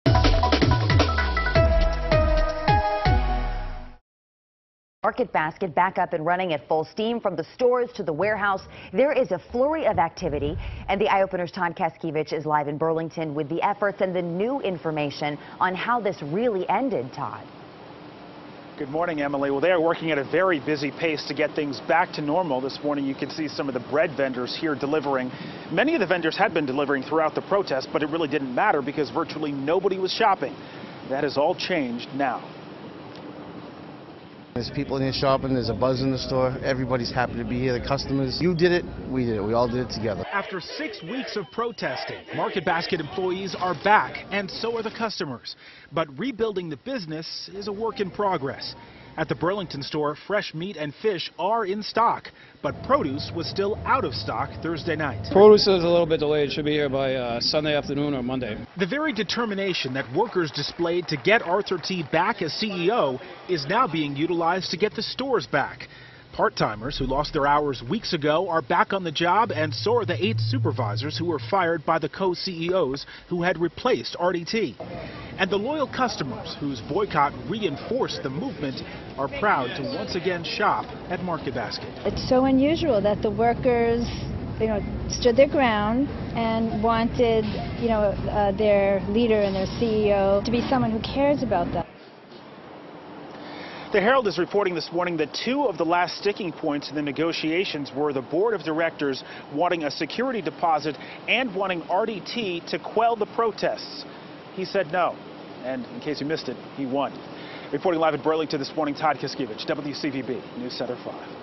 Rush on to stock Market Basket shelves
The rush is on to stock the shelves at Market Basket stores.